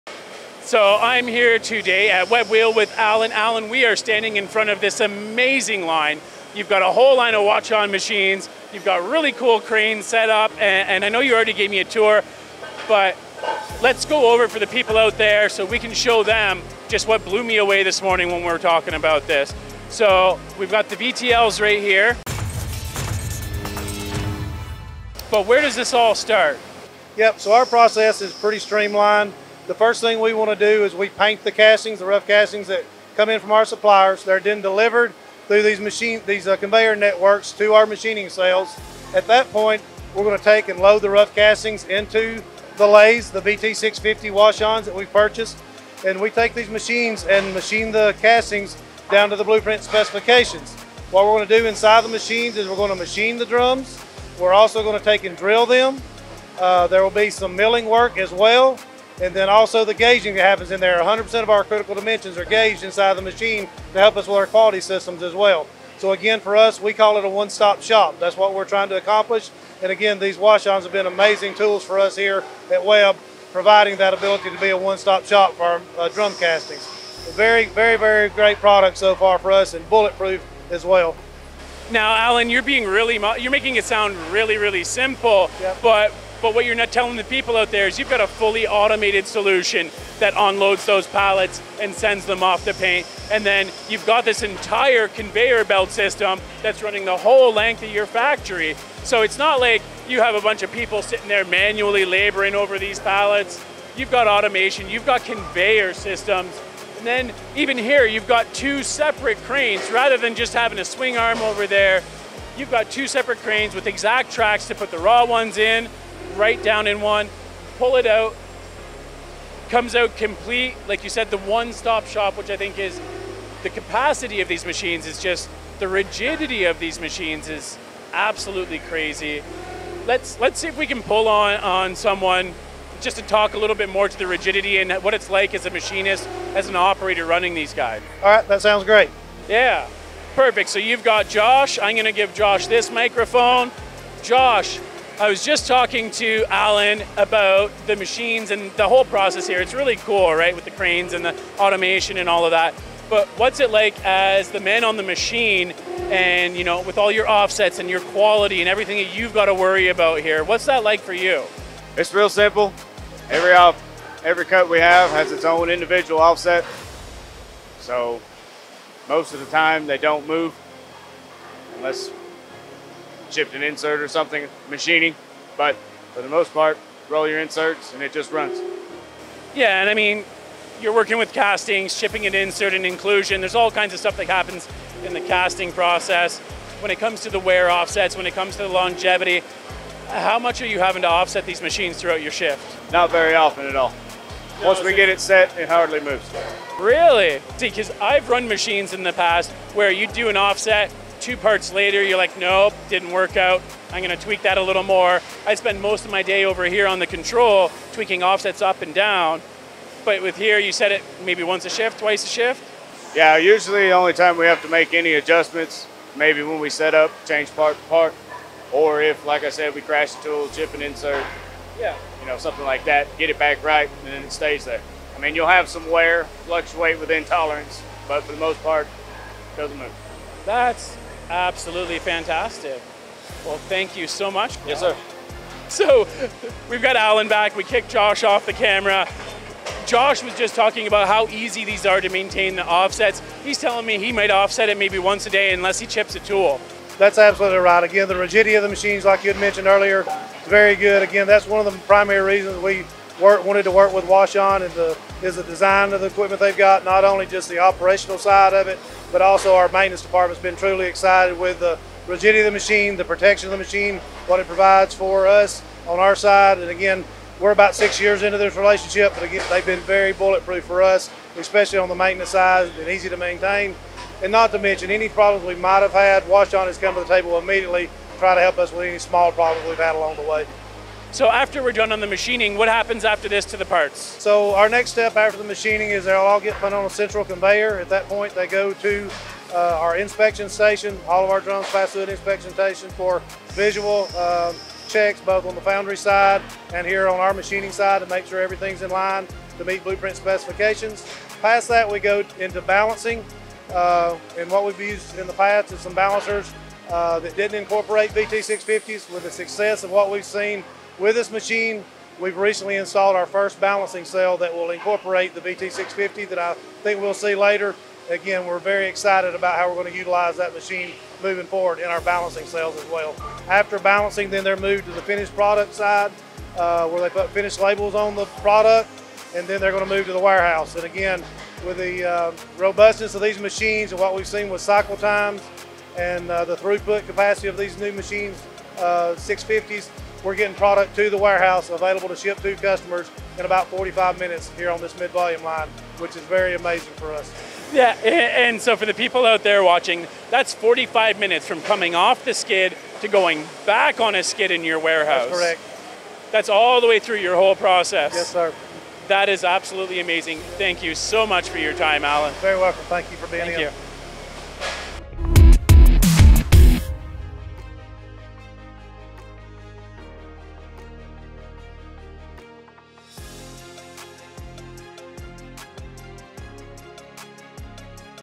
How did they achieve a “one-stop shop” in their machining process?
Webb Wheel Products have such a streamlined operation, they’re getting products to and from the warehouse in forty-five minutes!!

What’s their secret? An amazing line of Hwacheon machines that gives them some crazy capacity and rigidity!

Easy maintenance, simplified offsets, and a ridiculous throughput – no wonder their relationship with Hwacheon machines is six years strong. Tune in to learn more with MTDCNC’s Arthur Field and Webb Wheel Products’ Alan Quick and Josh Peeples!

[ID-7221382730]